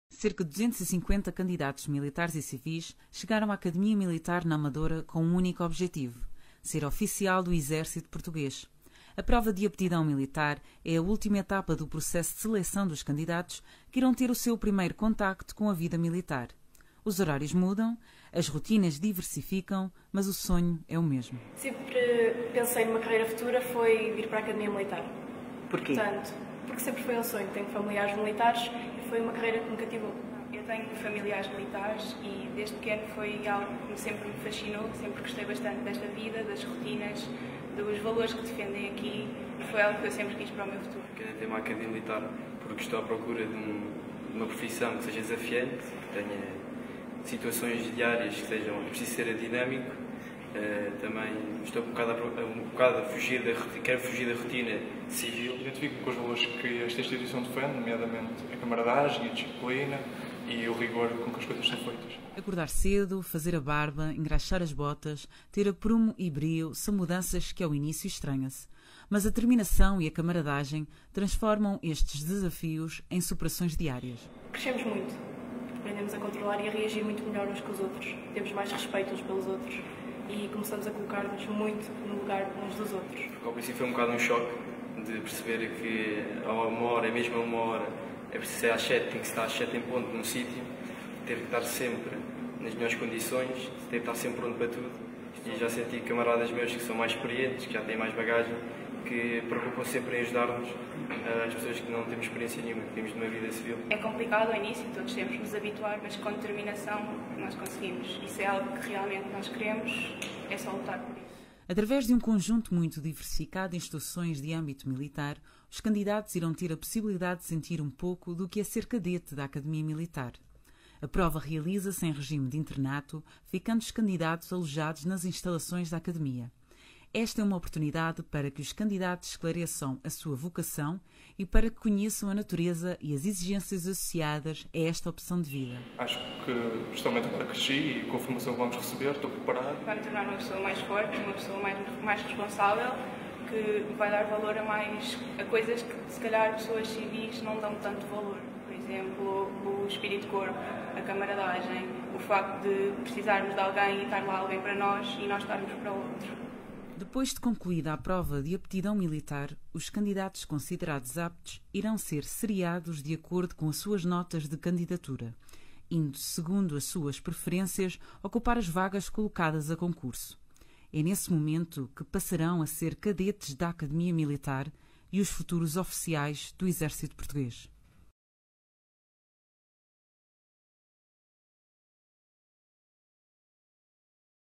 Prova de Aptidão Militar - Academia Militar
Um único sonho é partilhado por cerca de 250 jovens na Prova de Aptidão Militar.

Esta é a oportunidade final para confirmarem a sua vocação e conhecerem as exigências associadas à vida militar e, em especial, ao dia a dia de um Cadete da Academia Militar.

Conheça a motivação e a determinação dos nossos candidatos e boa sorte a todos.